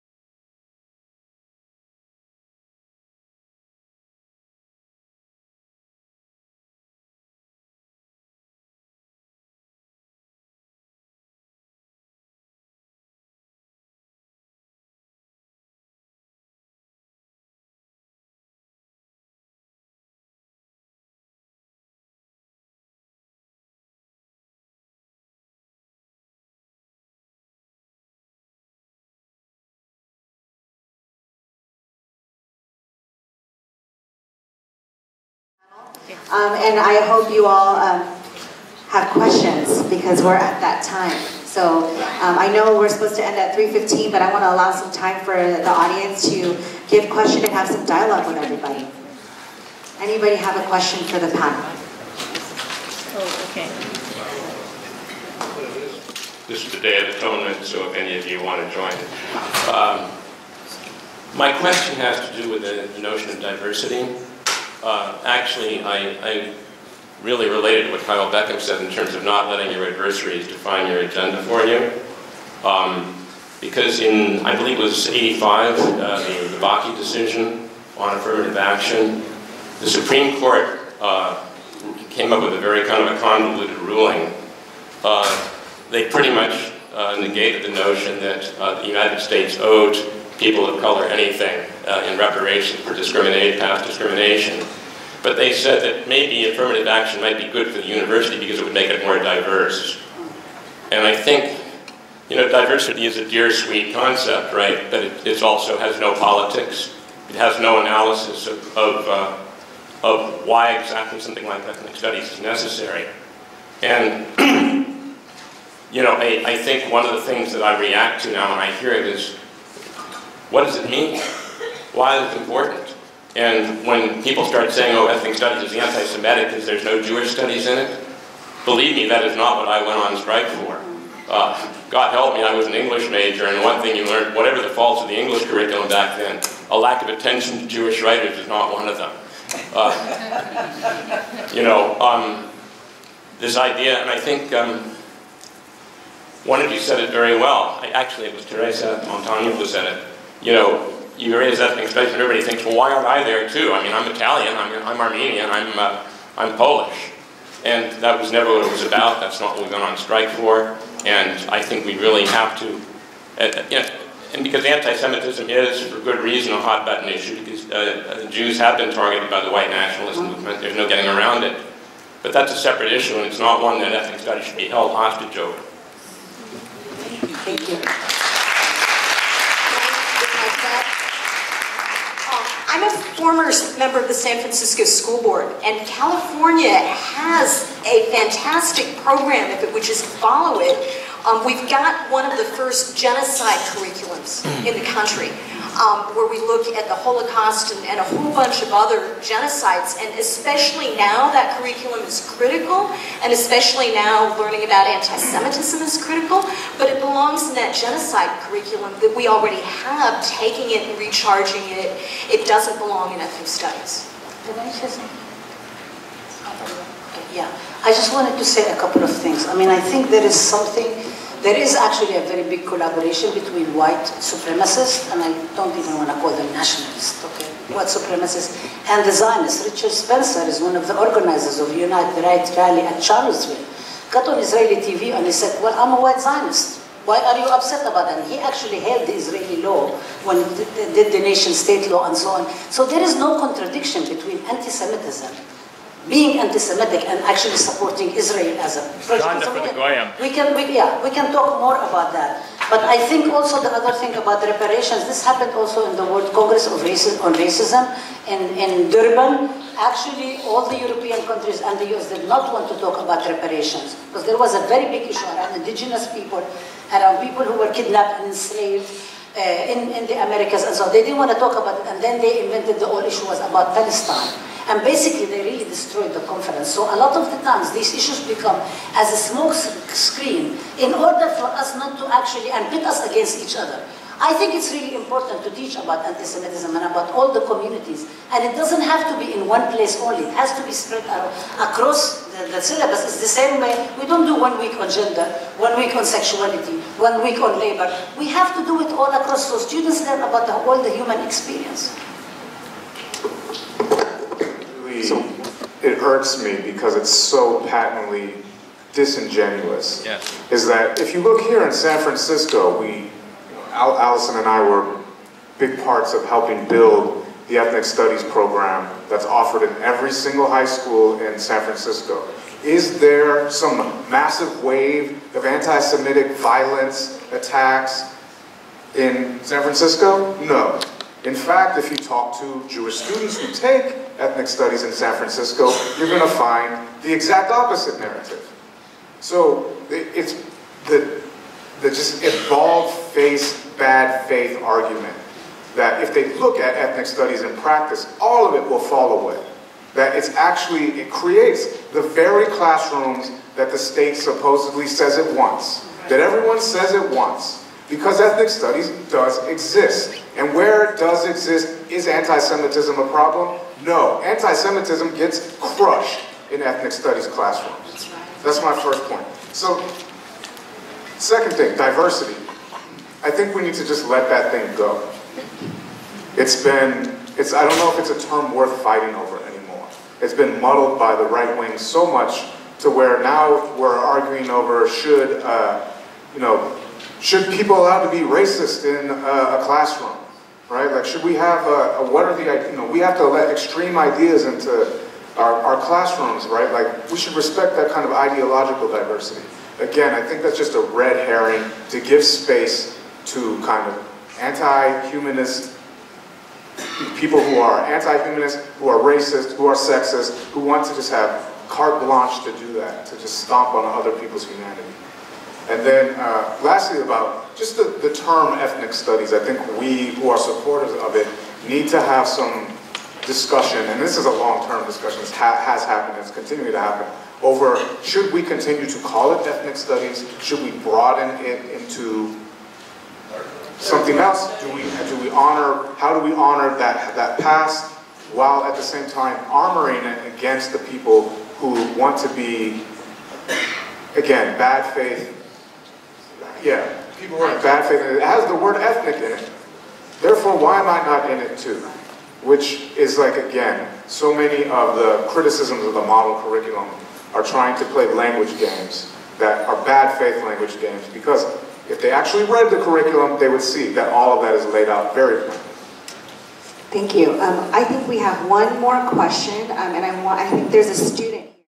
50th Anniversary Commemoration of the 1968-69 San Francisco State College Strike (Clip No. 57)
Clip No. 57:

Panel 2, Q & A --

Q & A -- Audience Engagement

Panel 2 Topic:

PRACTICING ETHNIC STUDIES IN EDUCATION

Panelists:

-- Arlene Sudaria Daus (Moderator), Director, Asian American &
   Pacific Islander Student Services

-- Allyson Tintiangco-Cubales, Asian American Studies, CA
   Commission for Ethnic Studies Curriculum

-- Theresa Montano, Chicana/o Studies, CSU Northridge

-- Kyle Beckham, Former Teacher of Ethnic Studies @ Mission High
   School in San Francisco; Current Lecturer @ SF State Umiversity

-- Professor Rabab Abdulhadi, Director of Arab and Muslim Ethnicities and
   Diaspora Studies in the College of Ethnic Studies @ SF State University

PRACTICING ETHNIC STUDIES:
PAST, PRESENT AND FUTURE

Jack Adams Hall (Cesar Chavez Student Center)
San Francisco State University
1600 Holloway Avenue